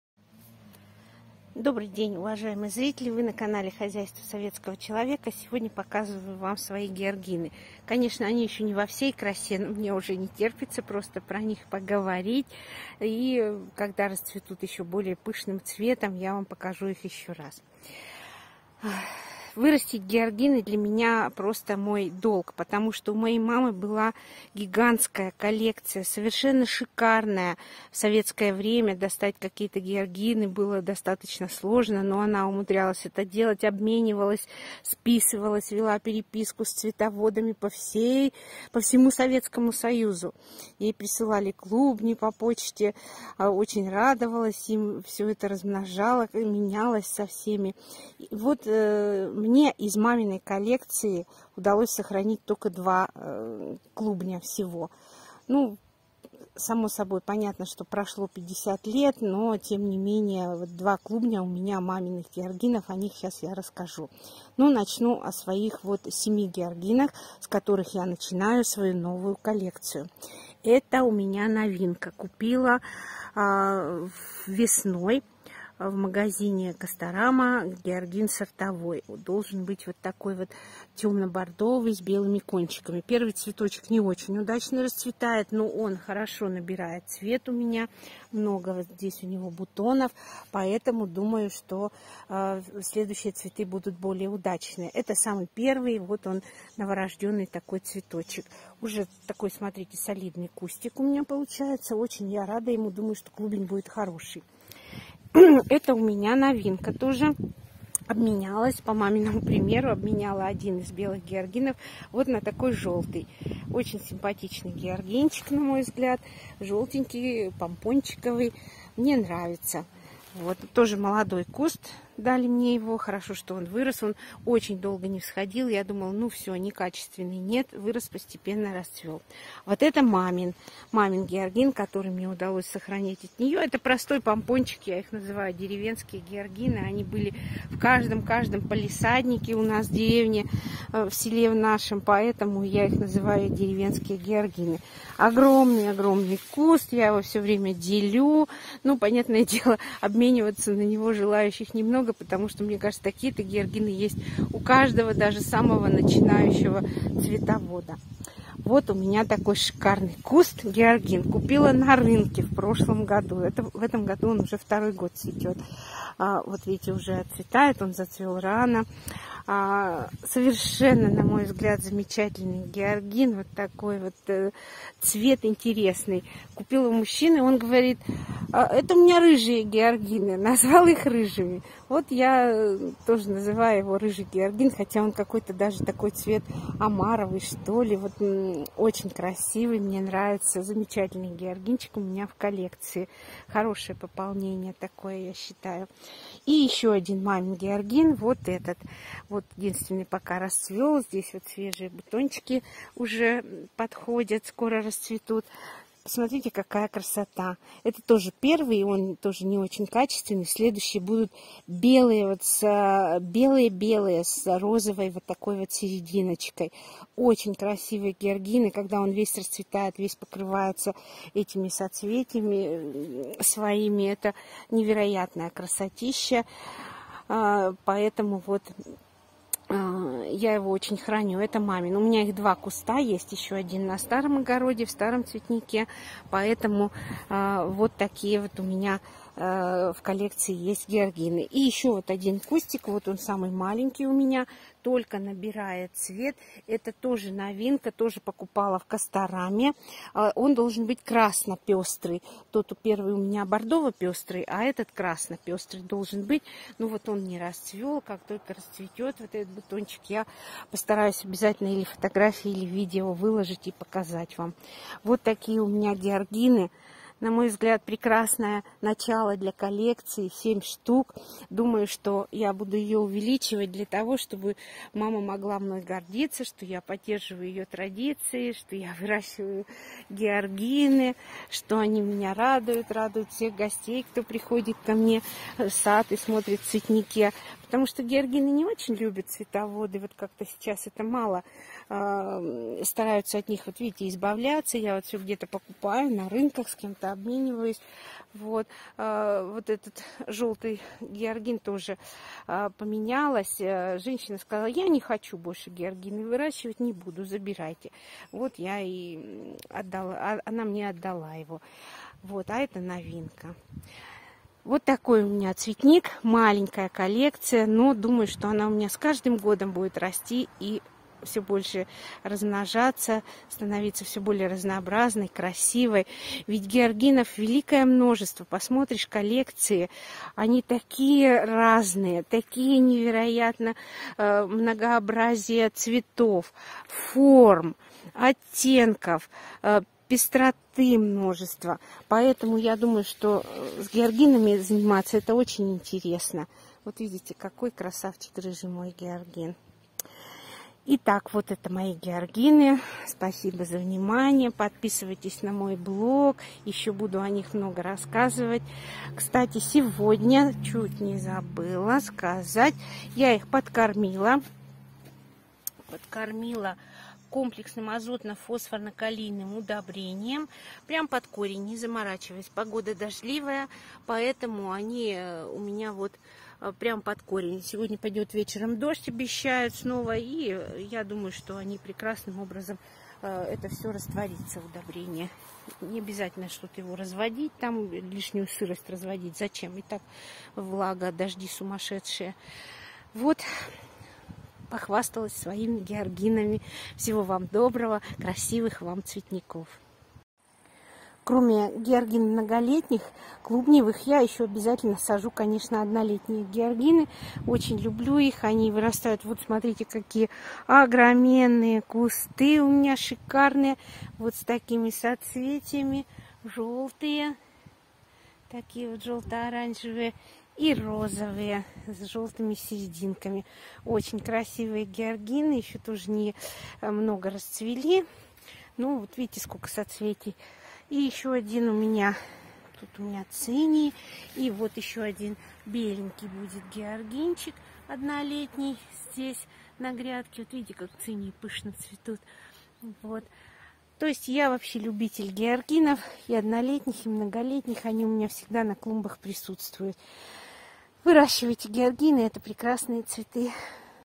Георгины - начинаю собирать новую коллекцию в память о маме, у которой была замечательная подборка
Я очень люблю георгины. Это яркие, сочные цветы, радующие нас с июля по октябрь.
У моей мамы была великолепная коллекция, хотя в советское время купить их было не так просто.
Мне удалось сохранить всего 2 клубня после мамы.
И вот теперь, когда у меня появилась возможность обеспечить прекрасные условия в сухом подвале для зимовки георгинов, я решила в память о маме  возродить новую, большую коллекцию этих цветов. Пока у меня 7 видов, но, надеюсь, что их станет много больше.